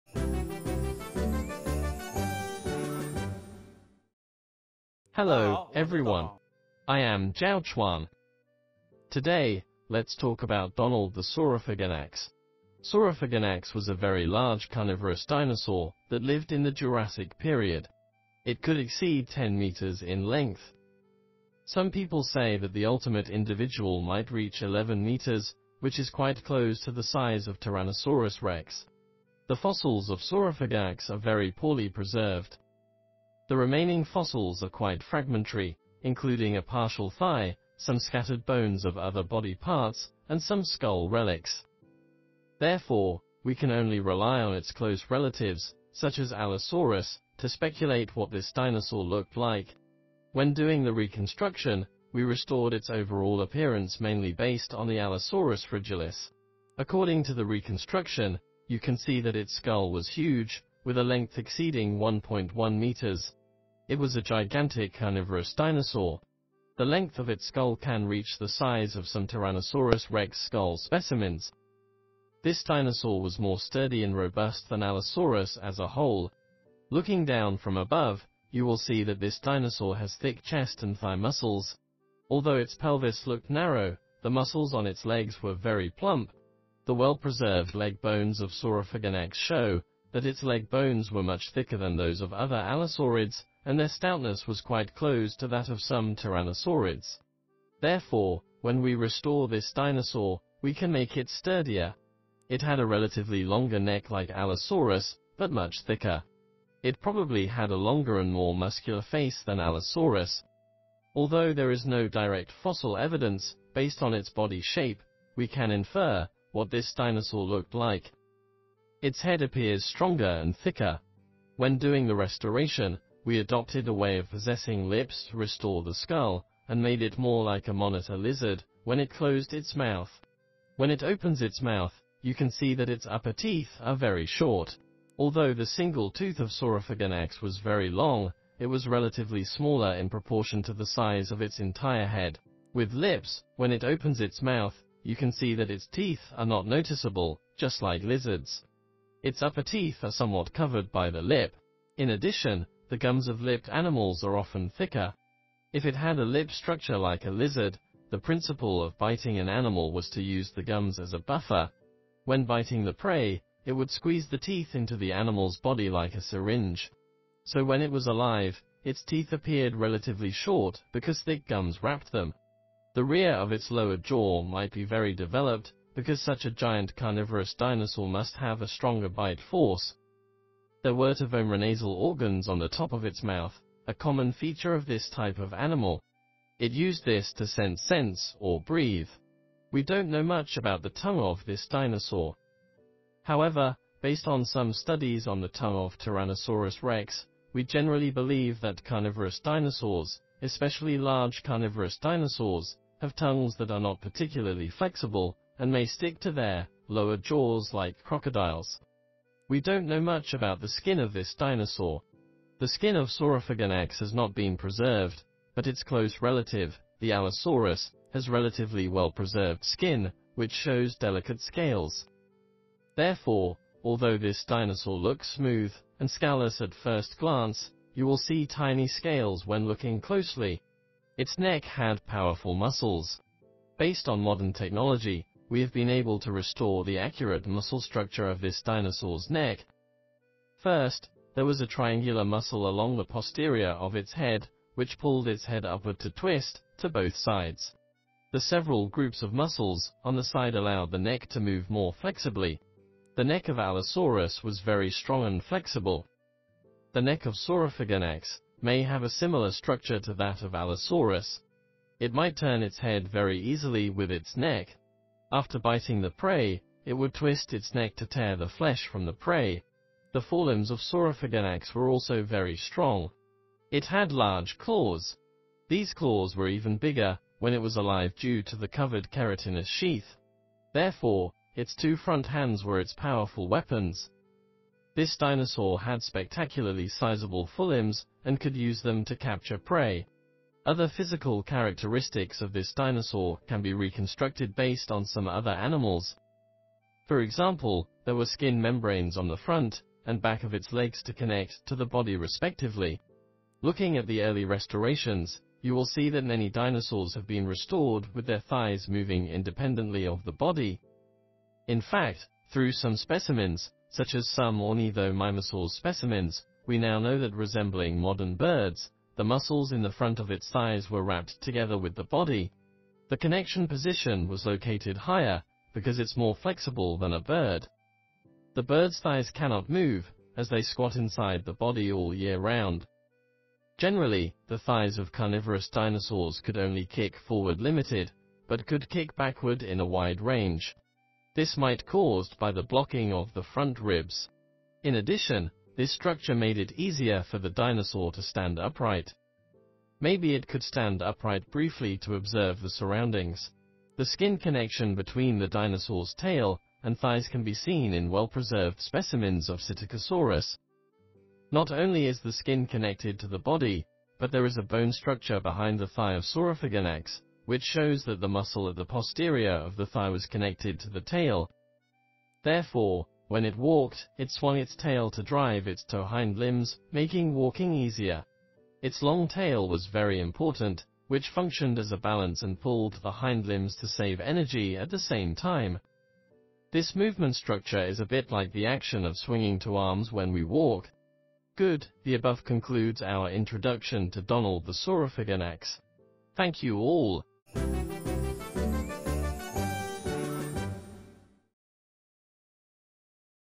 75 Donald the Saurophaganax | The Story of Creating Donald the Saurophaganax
Hi, everyone. Let’s get to know dinosaurs with ZHAO Chuang. In today’s video, he will talk about Donald the Saurophaganax, a large carnivorous dinosaur that lived in the Jurassic period. Its body length could exceed 10 meters with an ultimate length of up to 11 meters, which is quite close to the size of Tyrannosaurus rex. Check the video to find more.
The scientifically and artistically reconstructed model of Donald the Saurophaganax from the PNSO Prehistoric Animal Models series is available from PNSO online stores.